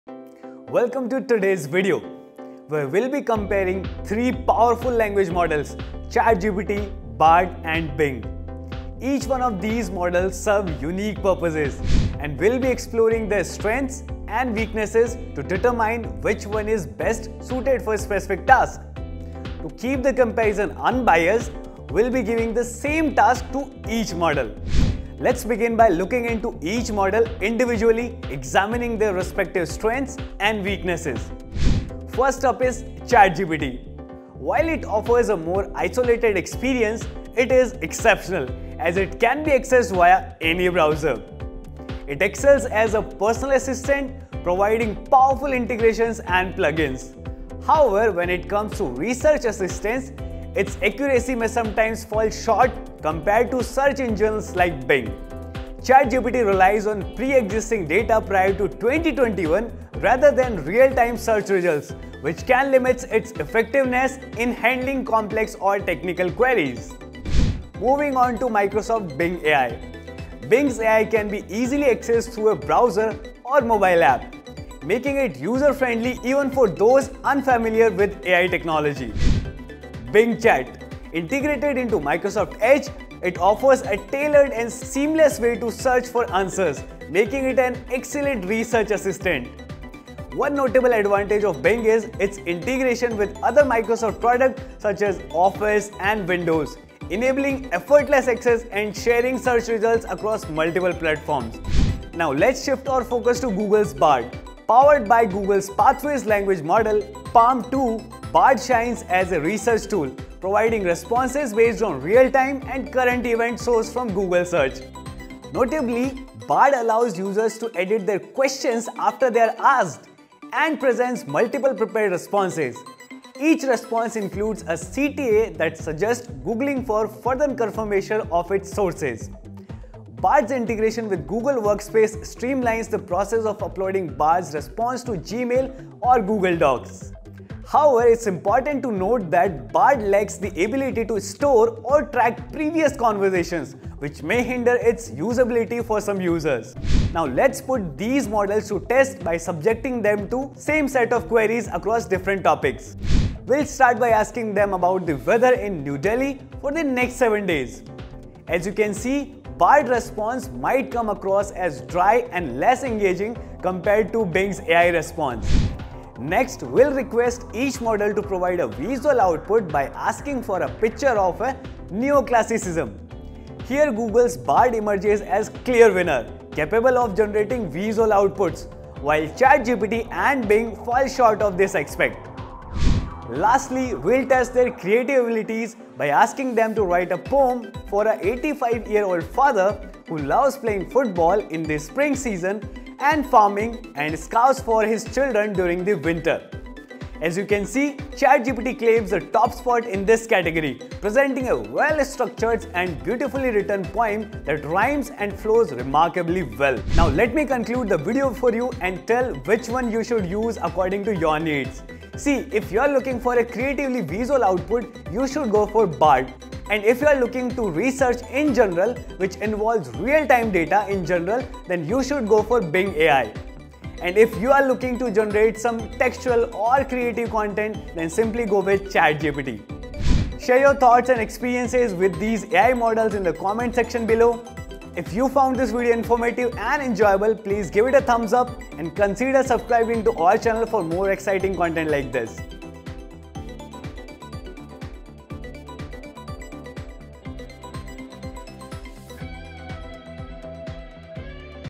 Battle of AI Language Models: ChatGPT vs Bing vs Bard
In this captivating showdown, we witness a clash of AI language models - ChatGPT, Bing, and Bard. Join us as we delve into the depths of these powerful models and compare their abilities, strengths, and weaknesses.

Discover the incredible capabilities of ChatGPT, an advanced language model developed by OpenAI. Explore its vast knowledge and impressive natural language processing skills that enable it to understand and generate human-like responses.

Next up, we have Bing, the popular search engine by Microsoft. Dive into the world of Bing's AI language model and explore how it handles queries, retrieves information, and assists users in finding the most relevant search results.

Last but not least, we introduce Bard, a cutting-edge language model that has gained recognition for its creative storytelling abilities. Witness the magic of Bard's narrative generation as it weaves captivating tales and engages users with its unique storytelling style.